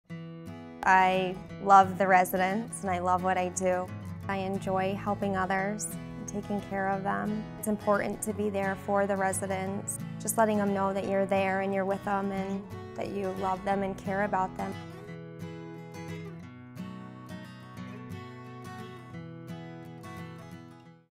Adirondack Health: Mercy Living Center - Whitney
"I love the residents, and I love what I do."

We offer a resident-centered care plan that is developed to meet specific physical, nutritional & psycho-social needs. Within the first week of residency, residents and their families meet with staff to develop an individualized Care Plan. This enables our Care Teams to assist the resident according to their preferences, and encourage participation in those activities to maximize their quality of living.